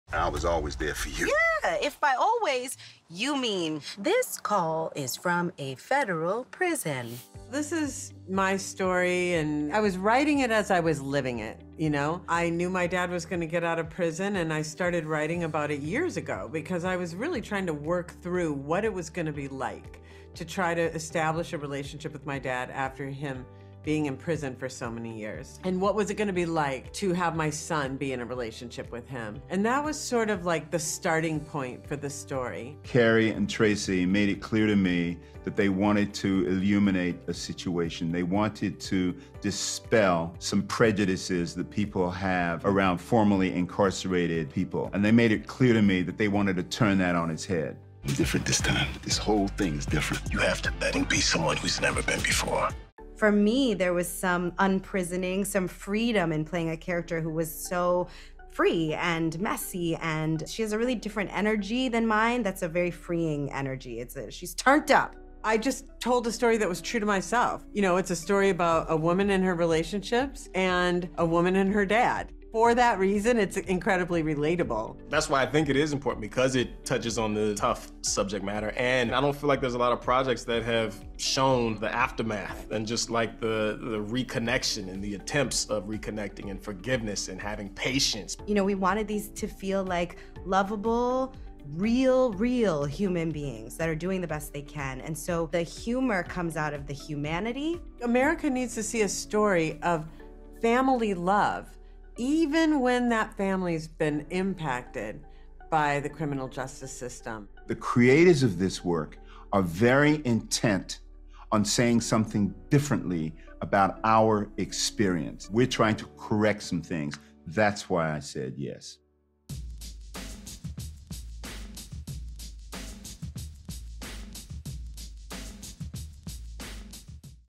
Why Kerry Washington And Delroy Lindo Want Everyone To Watch 'UnPrisoned'
HipHollywood chats with Kerry Washington, Delroy Lindo, Marque Richardson and creator Tracy McMillan about their new series, "UnPrisoned," streaming exclusively on Hulu.